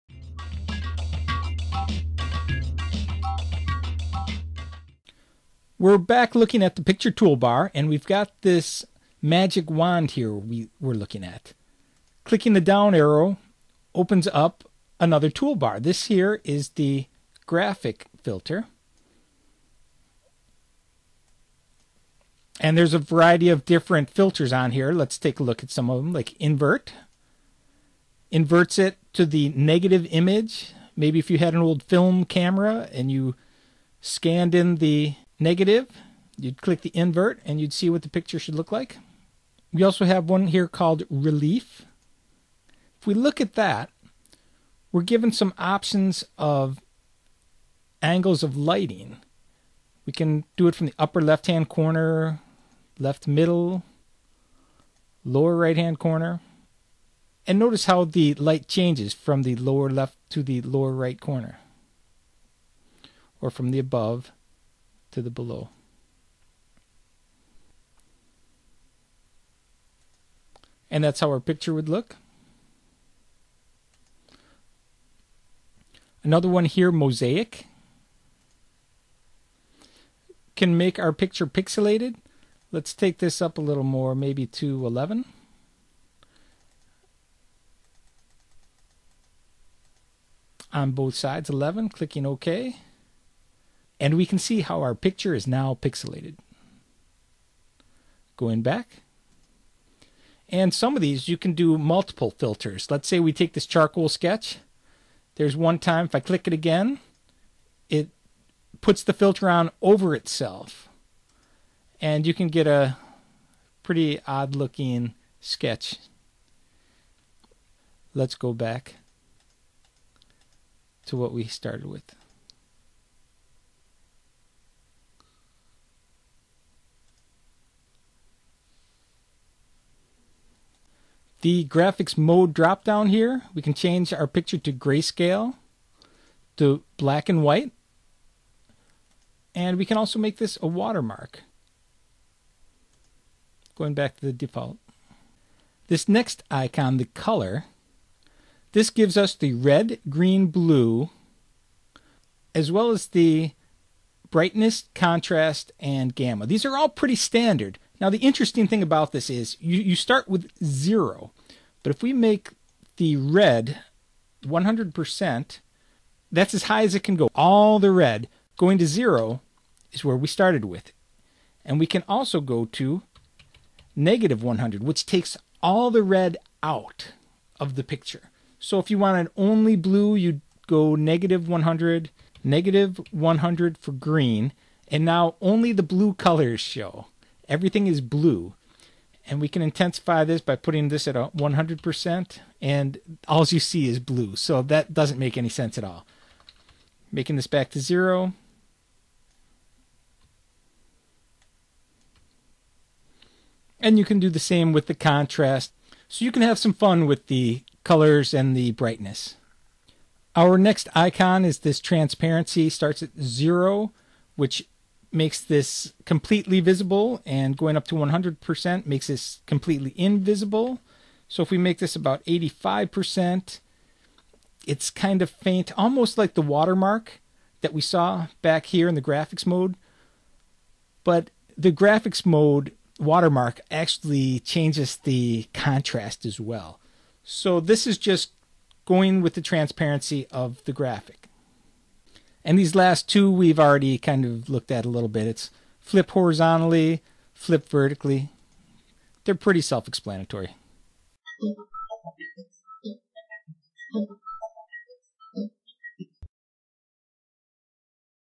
LibreOffice-Writer (67) Picture Graphics -- Part 3 Picture Toolbar
A look at some of the picture toolbar tools -- Filters, graphic mode, color, transparency and flip.